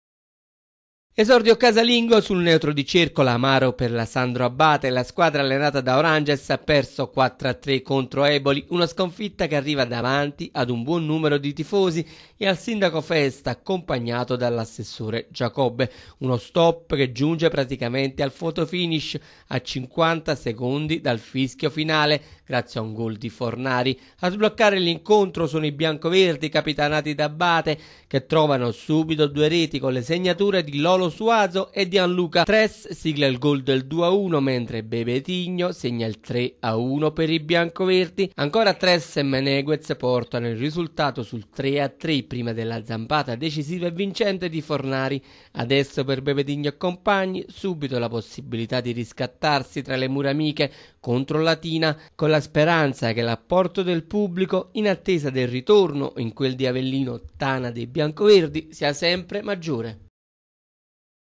Sandro Abate, esordio sfortunato
I ragazzi di Oranges vengono sconfitti per 4 a 3 a pochi secondi dalla fine.